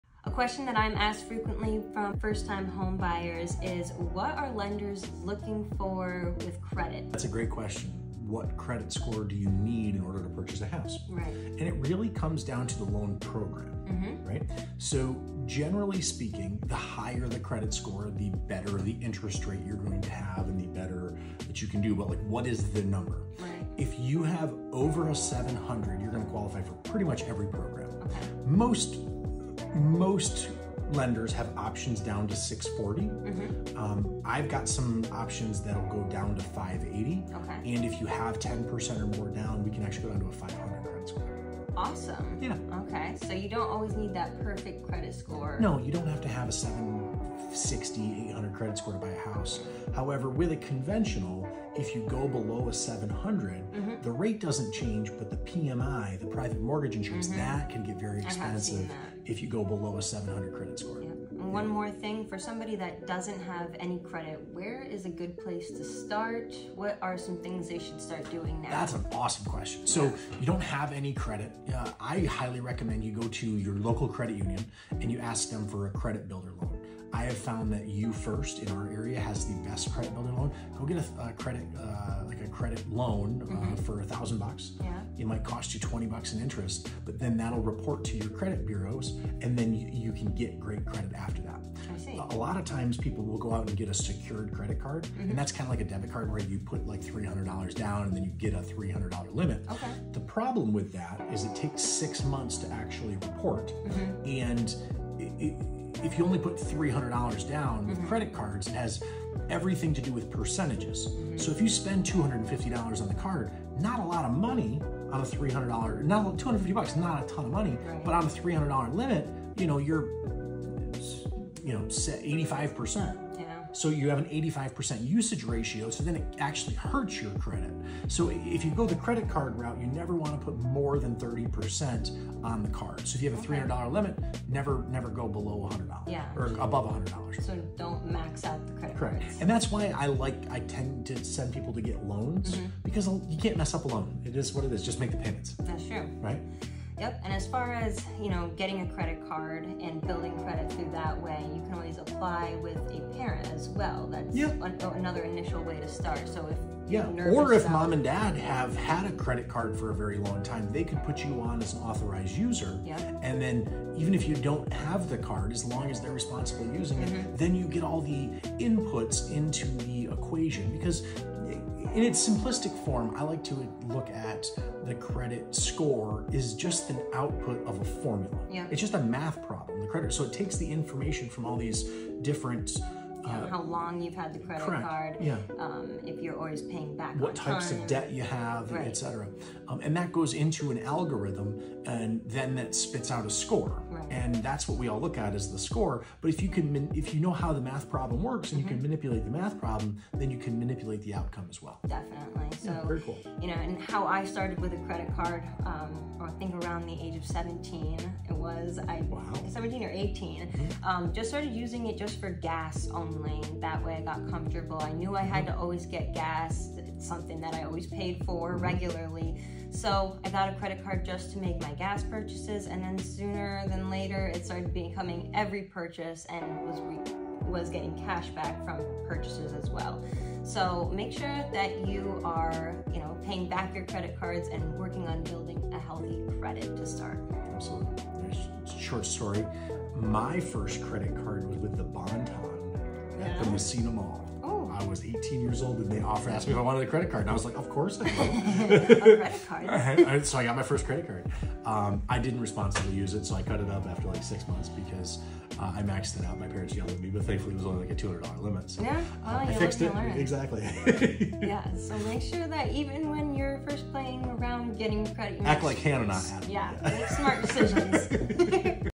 What credit score do you need to buy a home?
I am Hannah Wilson, a real estate agent in Plattsburgh, New York. If you have been wondering what your credit needs to look like to purchase a home, listen up as lender Adam Crosley at Crown Home Mortgage ￼spills the tea on credit scores.